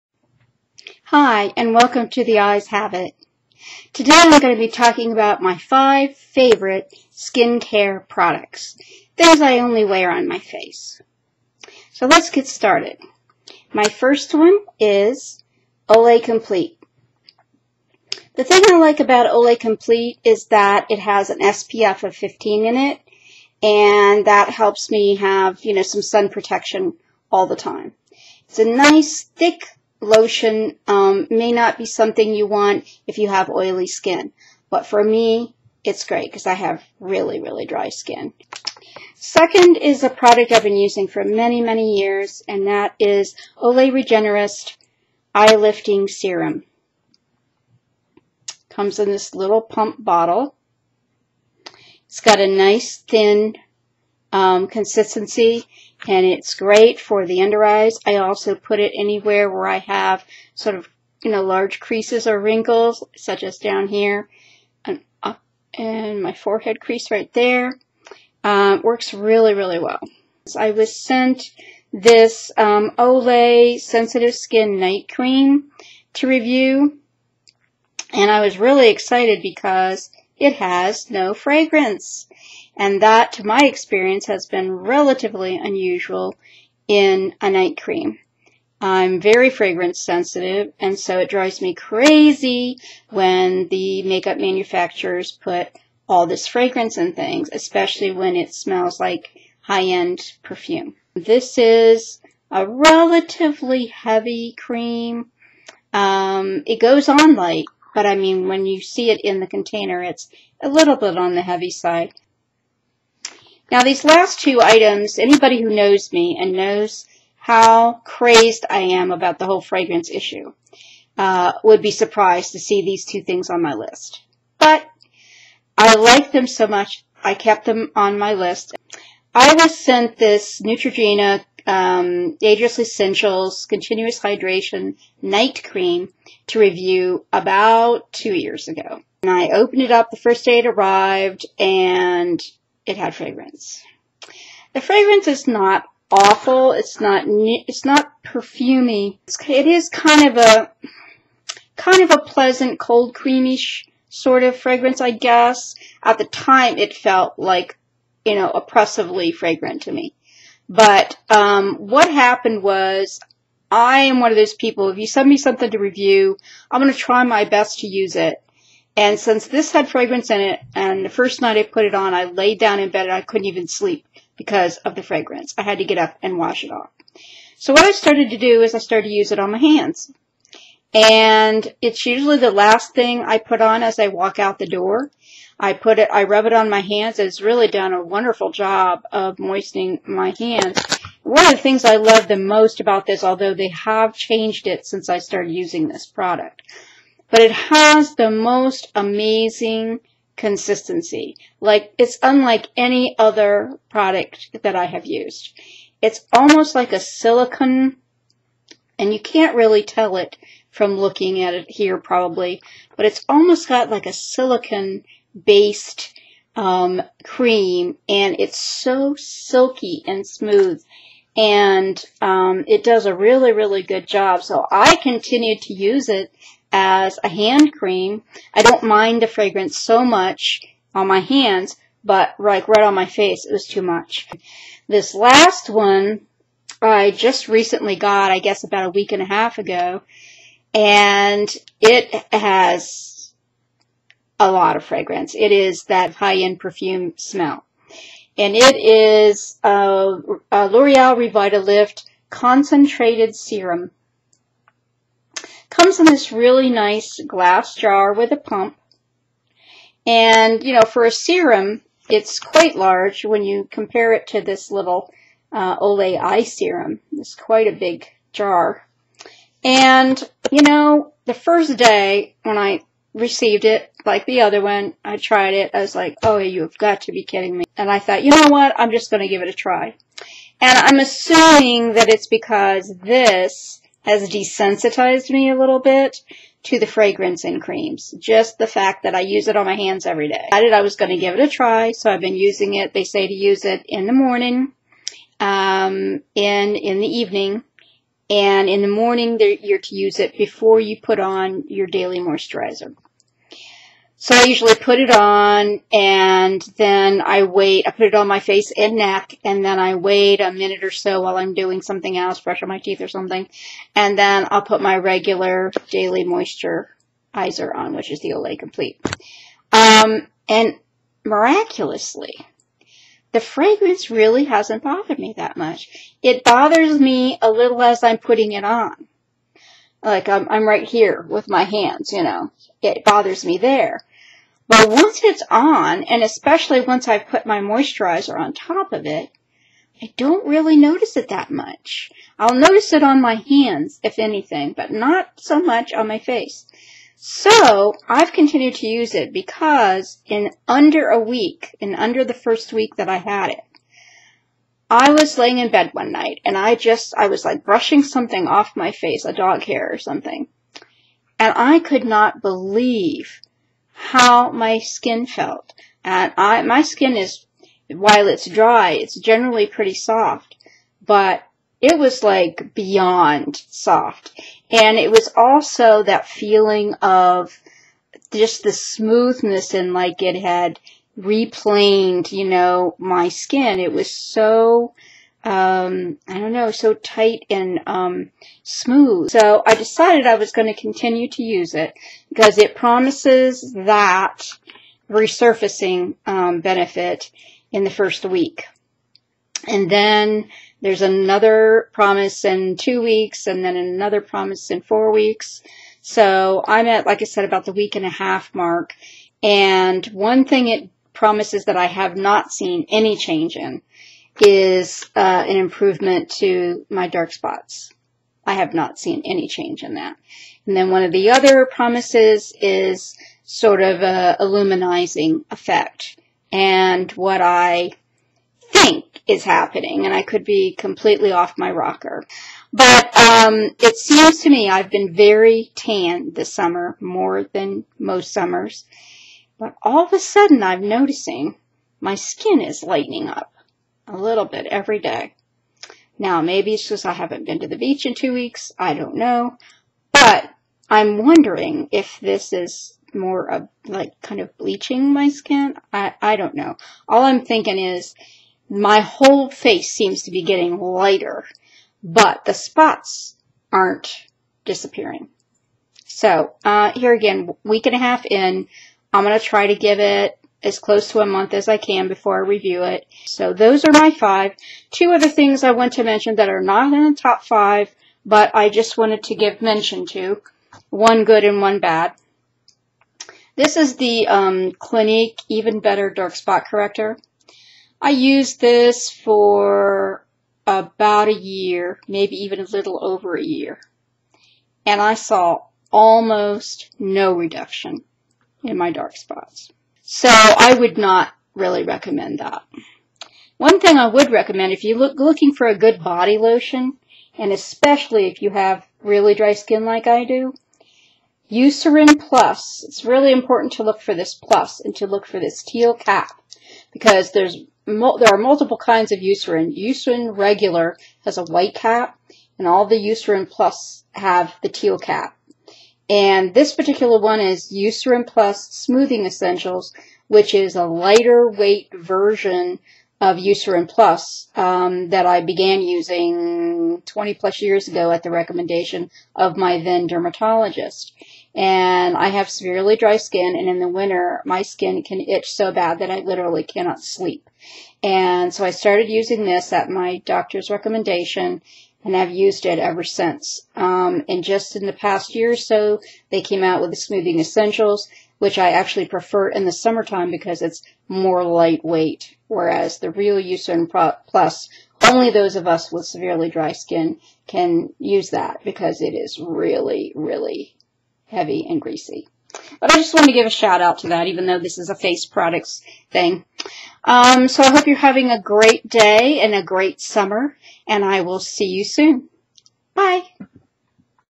Five Fave Skin Care Products (Face)
In this video, I show you my five favorite skin care products for the face. I used a new webcam for this video and the sound is awful. My apologies.

The products I talk about in this video are:

1) Olay Complete daily moisturizer
2) Olay Regenerist eye lifting serum
3) Olay Sensitive Skin Night Cream
4) Neutrogena Ageless Essentials Continuous Hydration - Night
5) L'Oreal Revitalift Concentrated Serum

HUGE apologies for the sound issues in this video. Still adjusting to the new webcam.